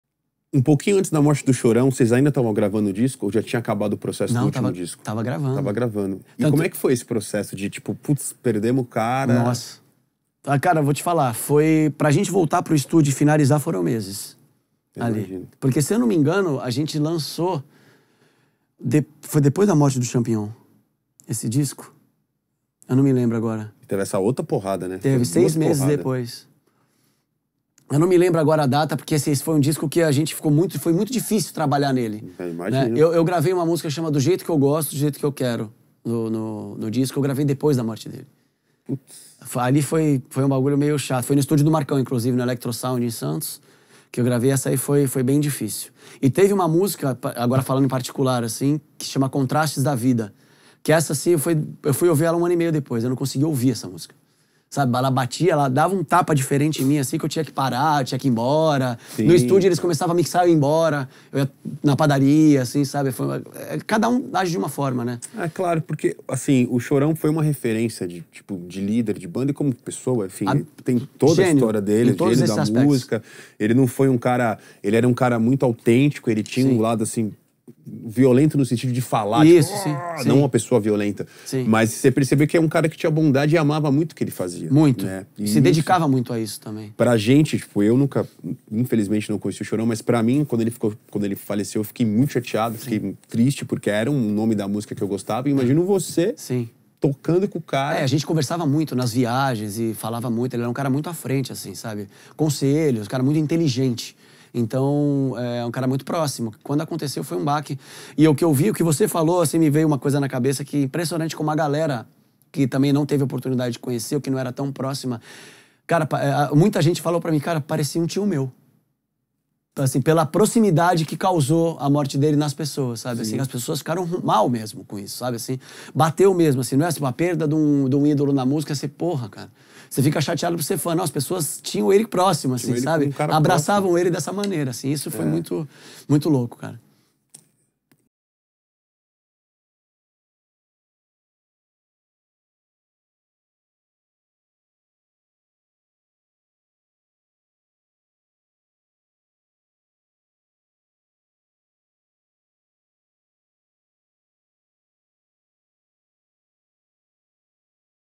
BRUNO GRAVETO fala sobre a morte do Chorão do Charlie Brown Jr | Braba Cortes
Nestre corte da entrevista com o baterista Bruno Graveto, ex-integrante da banda Charlie Brown Jr., ele compartilha suas lembranças e sentimentos sobre a trágica morte do icônico vocalista Chorão.

Feel Good Pads
Joel Drum Tech
IP&T - Giba Favery
Equipe BRABA:
► Helinho Ishitani - Produção de Áudio
► Maurício Cesar - Produção Visual

Produzido por Gabriel Coelho, Rogério Vivaldo e Giba Favery